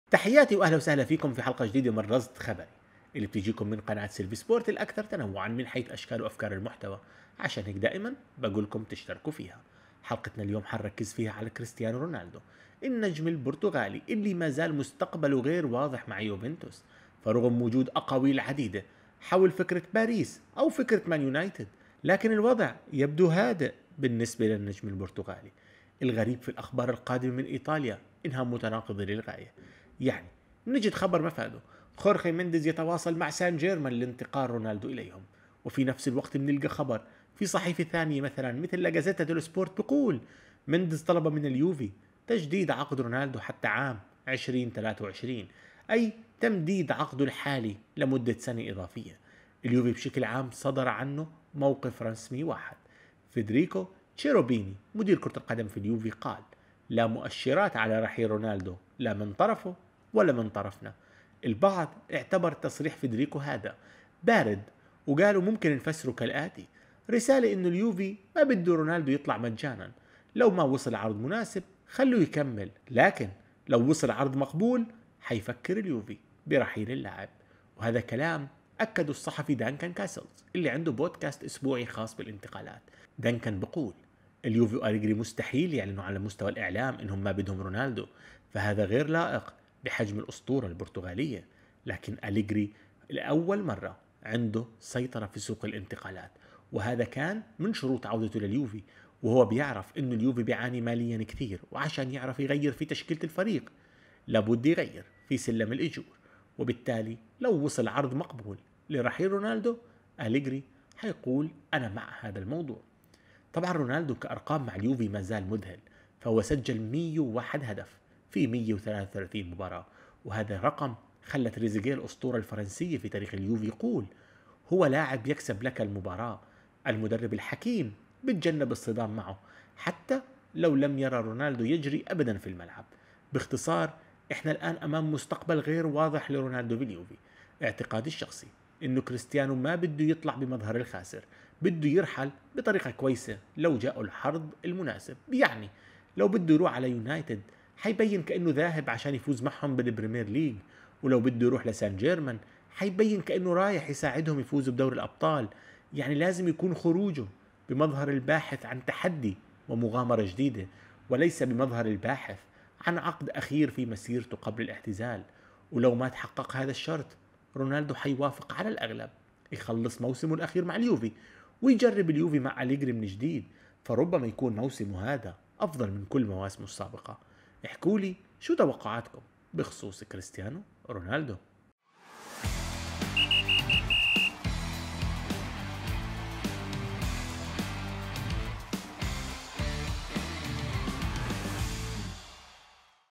مستقبل كريستيانو رونالدو في يوفنتوس غامض
مستقبل كريستيانو رونالدو في يوفنتوس ما زال غامضاً.

النجم البرتغالي رونالدو ما زال حائراً في يوفنتوس ..

حساباتي في شبكات التواصل:
تيلغرام :